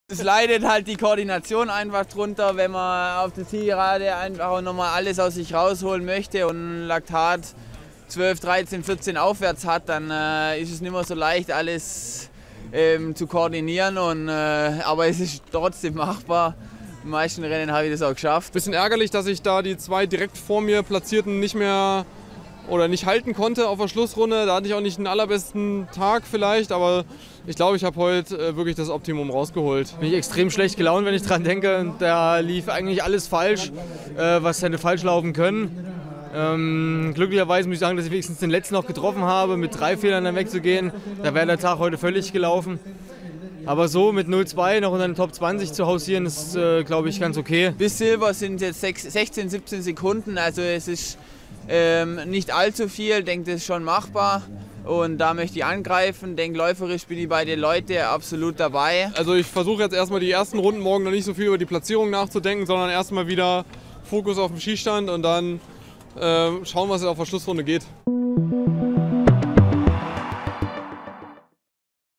Nach Sprint-Enttäuschung: "Silber in der Verfolgung machbar"
Hoffnungsträger Simon Schempp verpasste im Sprint bei der Biathlon-WM in Oslo deutlich die Medaillenränge. Auch Arnd Peiffer kam nicht über Platz sieben hinaus. Der Fokus liegt daher voll und ganz auf der Verfolgung am Sonntag.